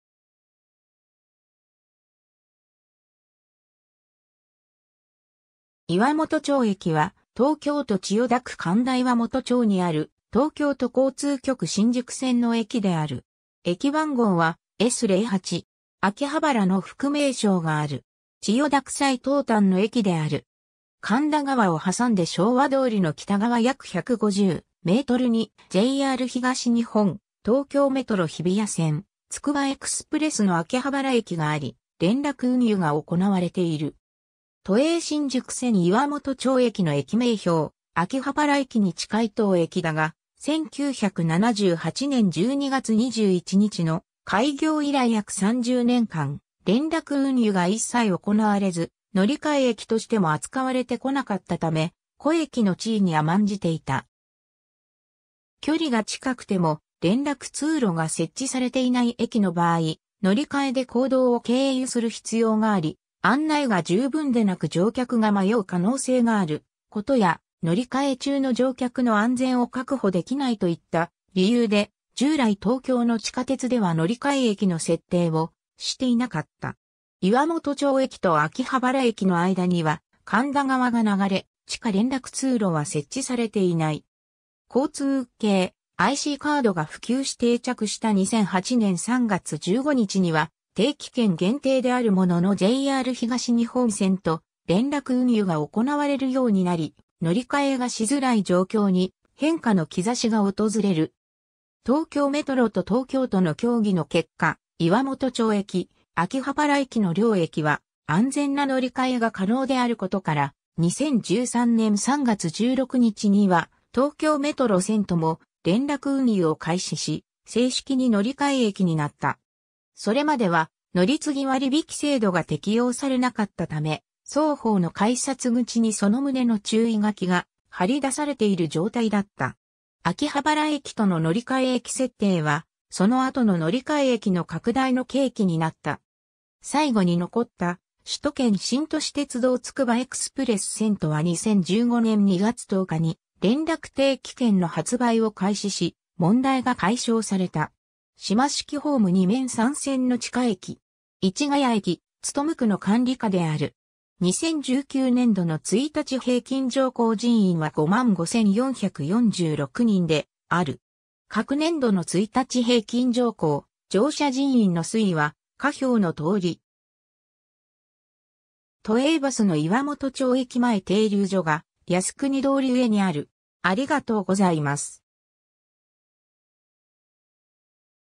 岩本町駅
岩本町駅（いわもとちょうえき）は、東京都千代田区神田岩本町にある、東京都交通局（都営地下鉄）新宿線の駅である。
駅番号はS 08。
「秋葉原」の副名称がある。
千代田区最東端の駅である。
 神田川を挟んで昭和通りの北側約150 mに、JR東日本・東京メトロ日比谷線・つくばエクスプレスの秋葉原駅があり、連絡運輸が行われている。
 都営新宿線岩本町駅の駅名標 秋葉原駅に近い当駅だが、1978年（昭和53年）12月21日の開業以来約30年間、連絡運輸が一切行われず、乗換駅としても扱われてこなかったため、小駅の地位に甘んじていた。
 距離が近くても連絡通路が設置されていない駅の場合、乗り換えで公道を経由する必要があり、案内が十分でなく乗客が迷う可能性があることや、乗り換え中の乗客の安全を確保できないといった理由で、従来東京の地下鉄では乗り換え駅の設定をしていなかった。
岩本町駅と秋葉原駅の間には神田川が流れ、地下連絡通路は設置されていない。
 交通系ICカードが普及し定着した2008年（平成20年）3月15日には、定期券限定であるもののJR東日本線と連絡運輸が行われるようになり、乗り換えがしづらい状況に変化の兆しが訪れる。
 東京メトロと東京都の協議の結果、岩本町駅、秋葉原駅の両駅は安全な乗り換えが可能であることから、2013年（平成25年）3月16日には東京メトロ線とも連絡運輸を開始し、正式に乗り換え駅になった。
それまでは乗り継ぎ割引制度が適用されなかったため、双方の改札口にその旨の注意書きが貼り出されている状態だった。
秋葉原駅との乗換駅設定は、その後の乗換駅の拡大の契機になった。
 最後に残った首都圏新都市鉄道つくばエクスプレス線とは2015年（平成27年）2月10日に連絡定期券の発売を開始し、問題が解消された。
 島式ホーム2面3線の地下駅。
市ヶ谷駅務区の管理下である。
 （出典：都営地下鉄:駅構内図） 2019年（令和元年）度の1日平均乗降人員は55,446人（乗車人員：27,752人、降車人員：27,694人）である。
 各年度の1日平均乗降・乗車人員の推移は下表の通り。
 都営バスの岩本町駅前停留所が靖国通り上にある。